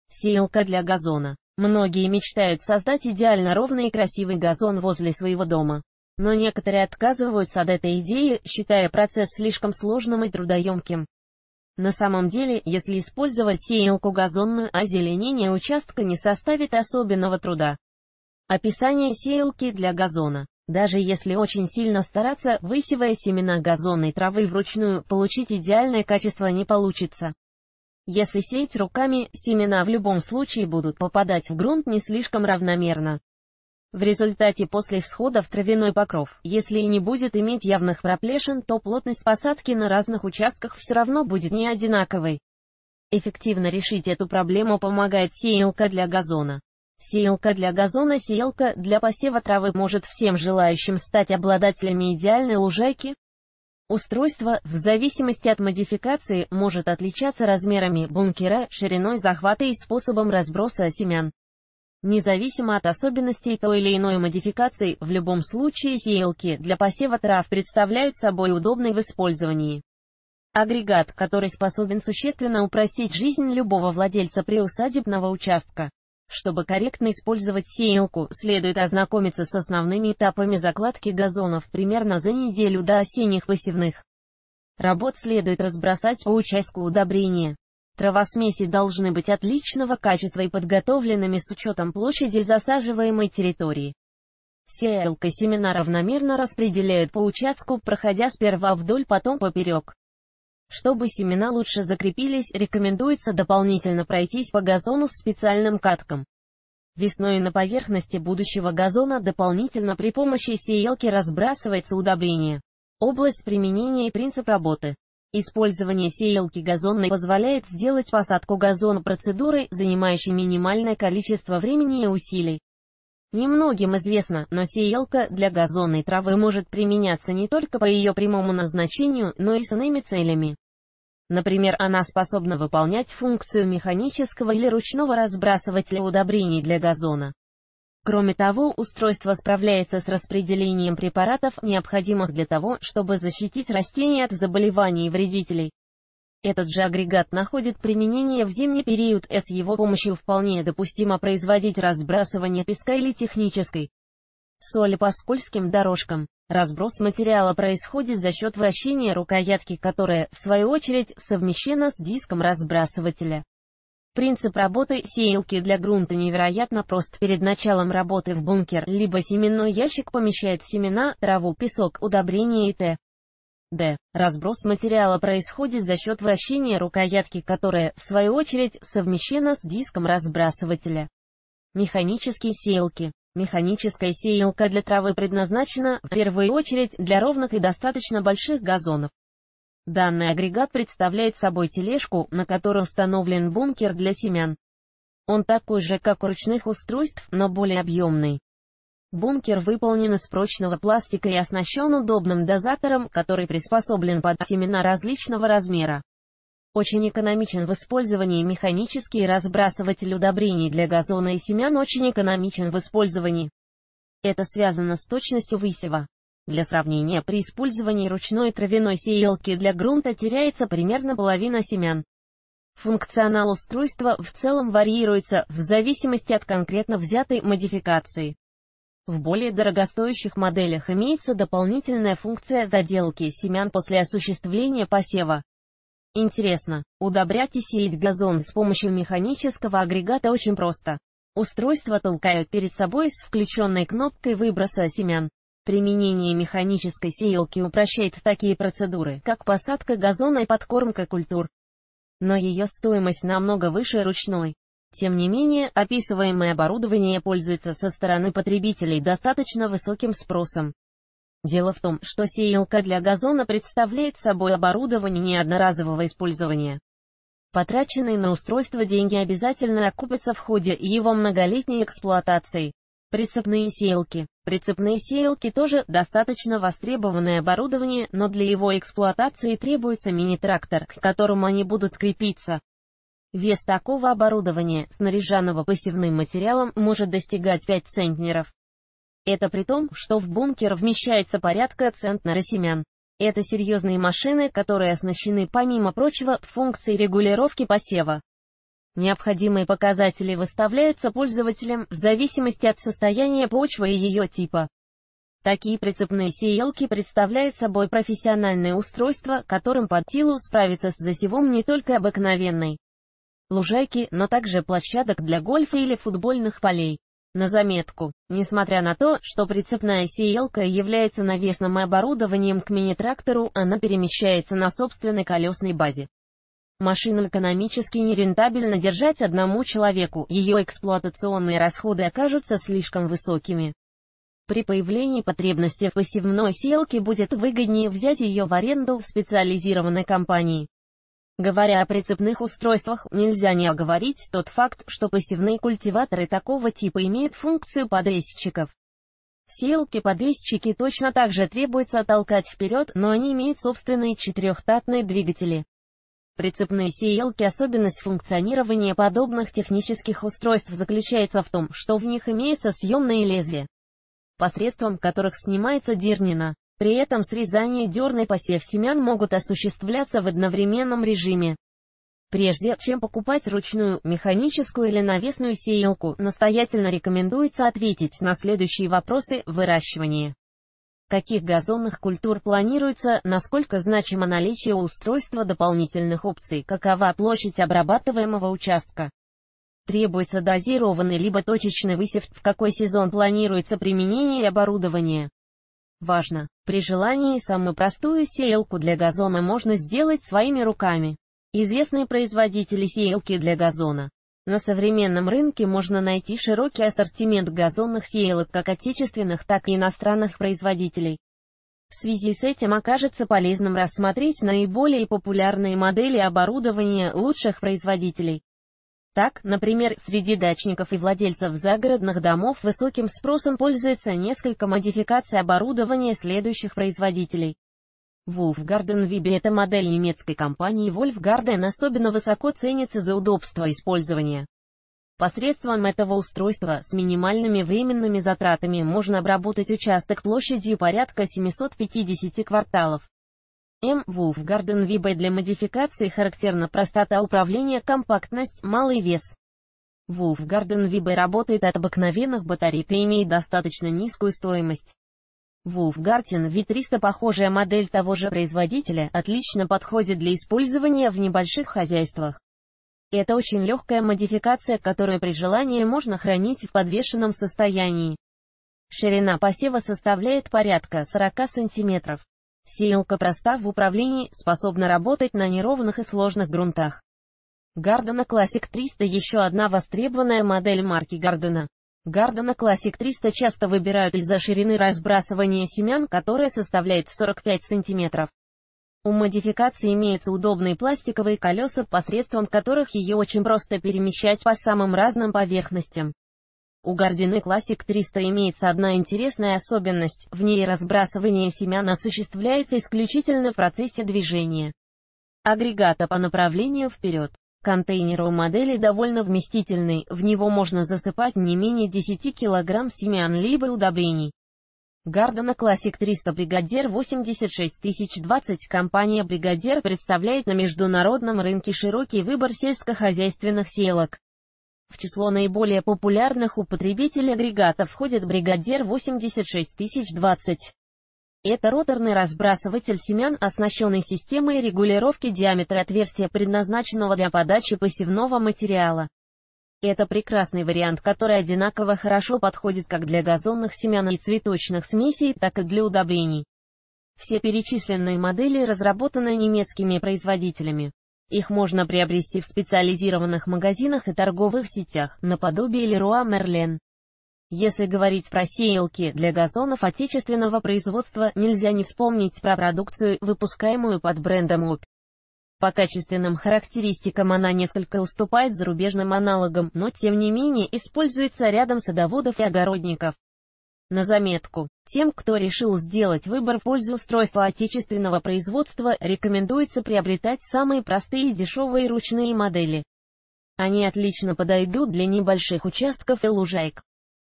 Сеялка для газона
Сеялка для газона – описание устройства, технических возможностей, достоинств и недостатков. Как сделать сеялку для газона своими руками в домашних условиях?